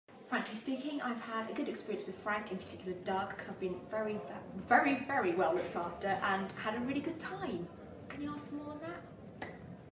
FRANK PR: MELINDA MESSENGER IS FRANKLY SPEAKING
Melinda Messenger tells us what she loves about Frank PR - with a special shout out for our very own Doug McPherson.